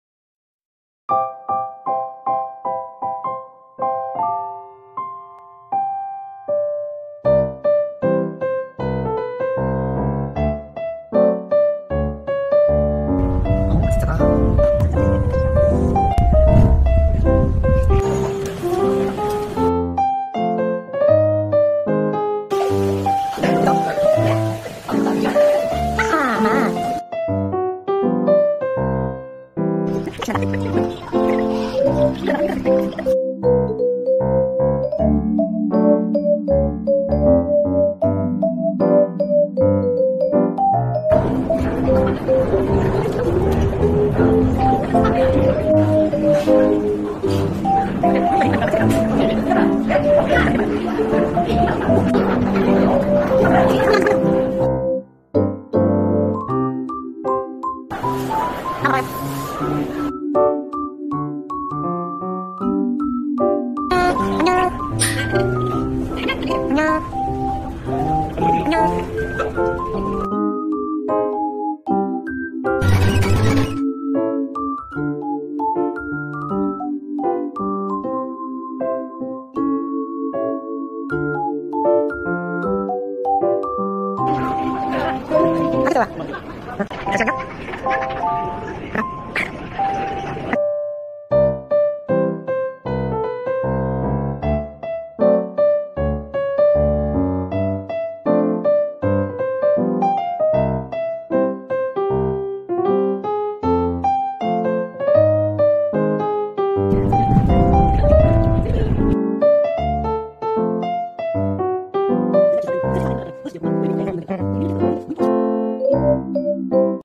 22.09.09. ~ 09.10. 바다의 추석맞이 여수여행 하이라이트
첫 추석을 맞아
할머니. 할아버지와 여수여행을 떠난 바다